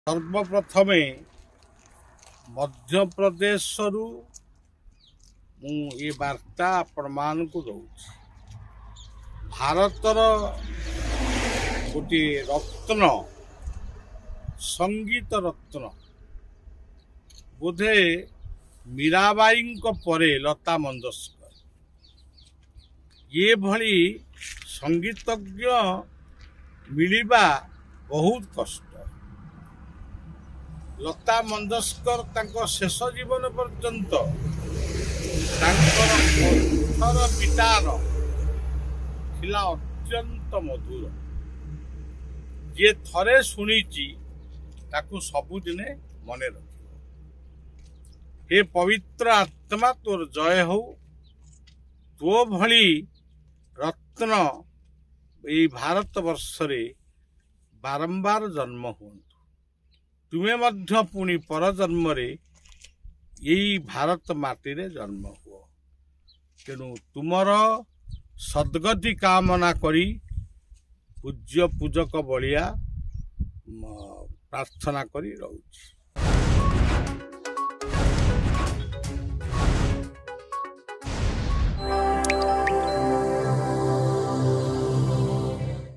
Lata Mangeshkar Passes away | Baba Balia Pays Tribute to Lata Didi
Baba Balia Pays Tribute to Veteran singer Lata Mangeshkar.